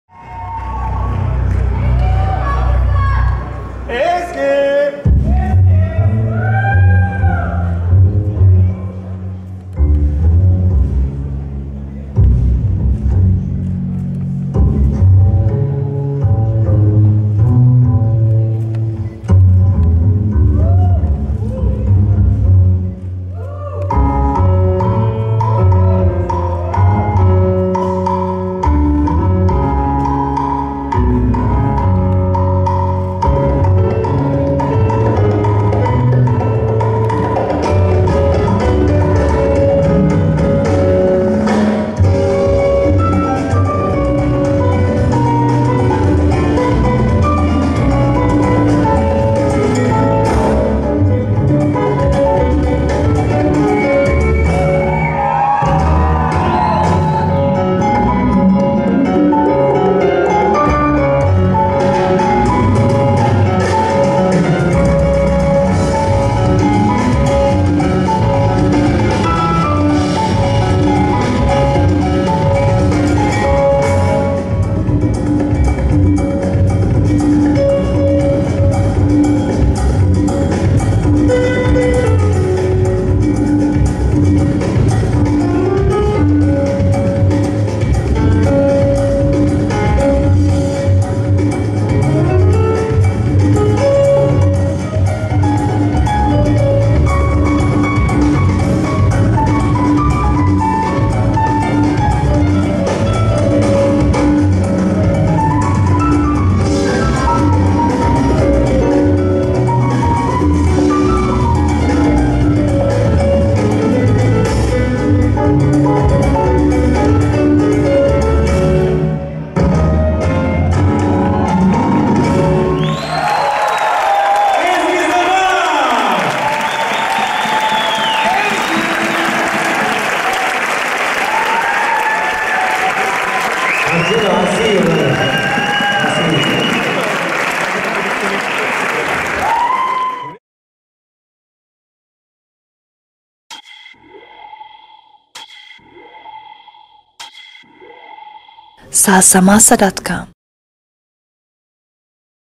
EZGİ ZAMAN SOLO DANCE PERFORMANCE | 1.İZMİR DANCE CONGRESS
Ezgi Zaman Solo Dance Performance - 1st Izmir International Dance Congress - Organizer : Ömer Ayaz​ - Artistic directors : Deniz Seven​ & Isabell Ksusha Holy​ - Festival - Venue : Kaya Termal Hotel, Balçova, İzmir - Video Record and Edit by : Murat Kasap